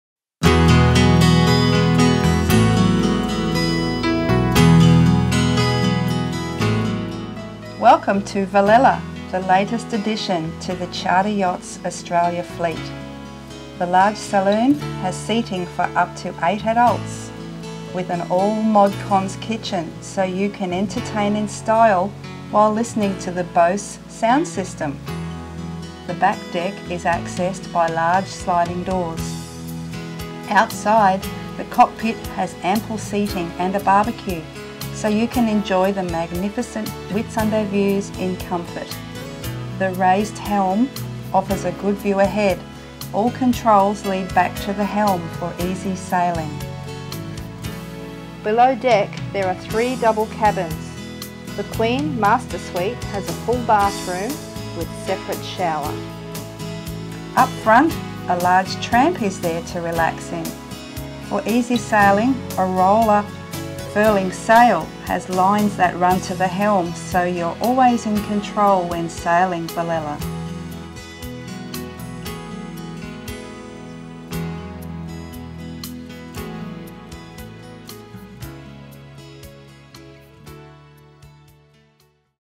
Velella - Leopard 39 Sailing Catamaran - Charter Yachts Australia
Cruise in style & luxury with the latest edition to the CYA fleet. Offering superior sailing performance expected from Leopard Catamarans, ‘Velella’ not only provides speed under sail, but comfort for you & your crew when you drop the anchor. Enjoy a functional saloon area featuring upstairs galley with plenty of storage for your provisions & table/seating for up to 8 adults.